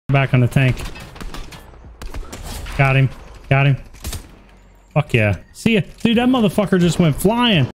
Guy went flying after I shot him!
Shot a guy after he launched out of a cannon and he went flying over the mountain. #